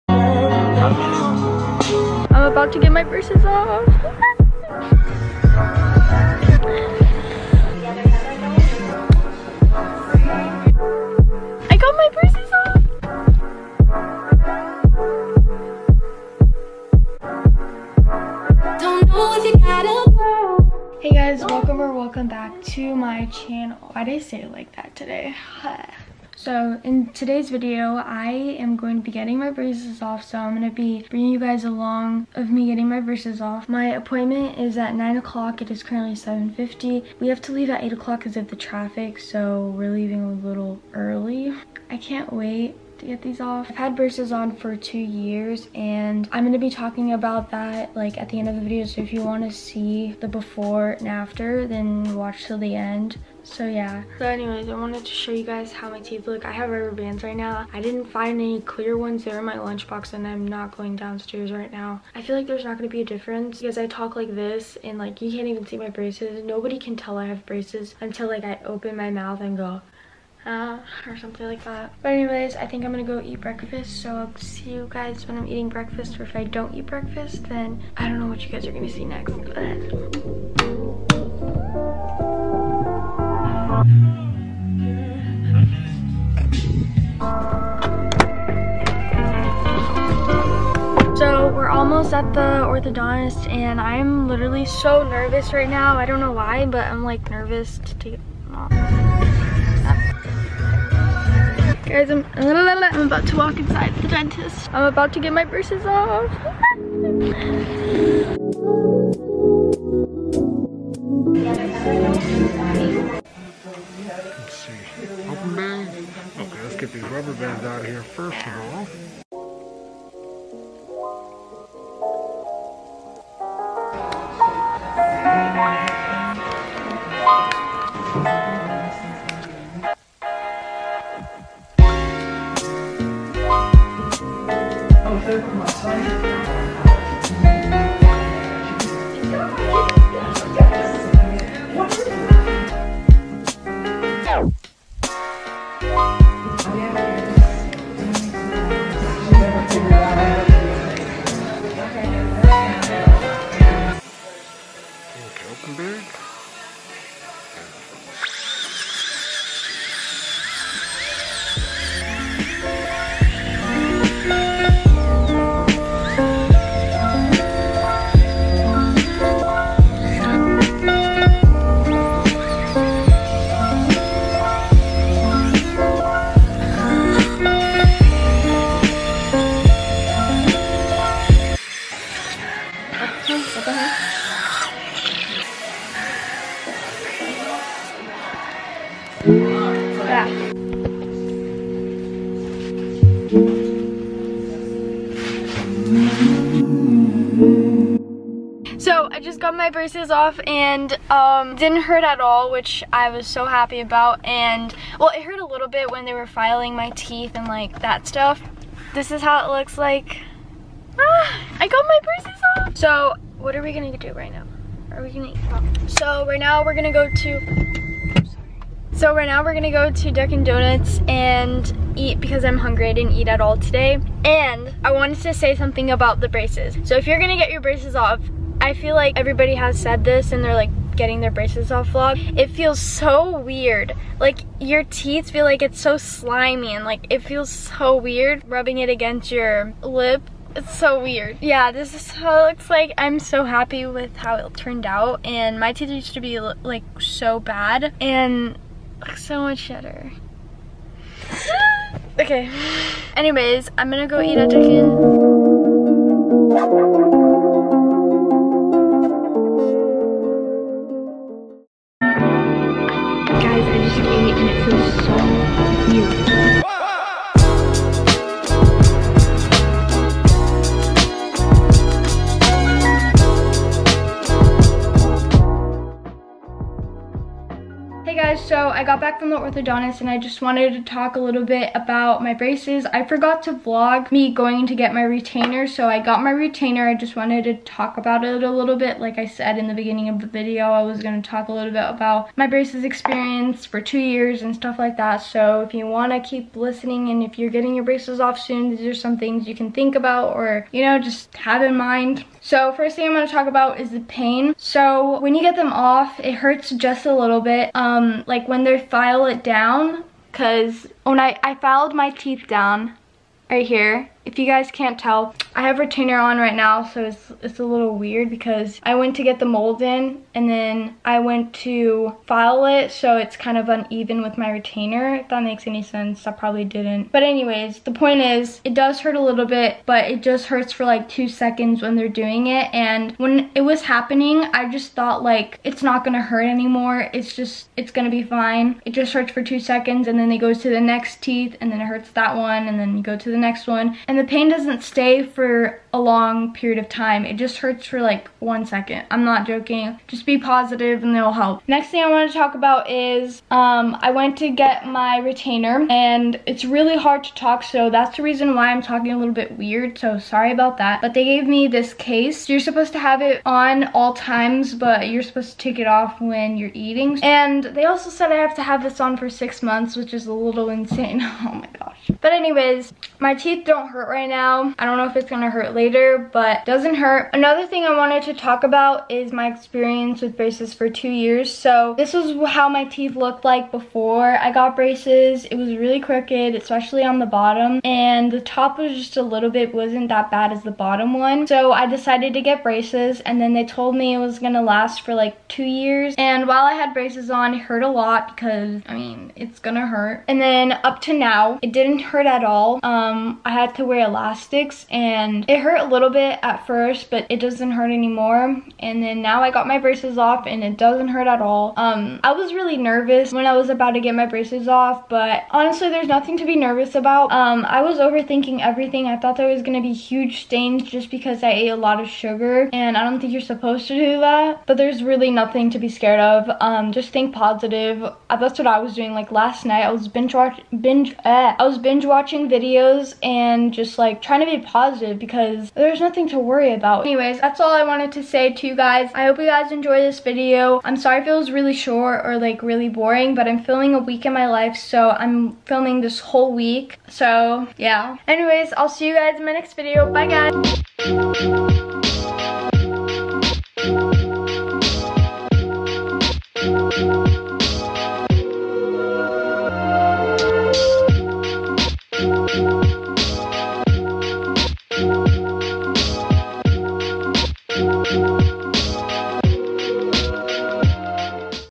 GETTING BRACES OFF 2019
so in this video you get to watch me get my braces off with me! I shared my experience with braces for 2 years!! I was super nervous but there's nothing to be nervous about;) I hope you guys enjoyed todays video!! if u have any questions about getting braces off or braces in general, dm me on instagram and I will try my best to reply back to u:) anyways I hope you guys enjoyed todays video and I will see you Sunday maybe;)

follow me☽
✰twitter➭emilyypaulichi
✰snapchat➭emilypaulichi
✰pinterest➭emilypaulichi
✰dote➭emilypaulichi

                                     as always i love you guys♡

                                                 emily paulichi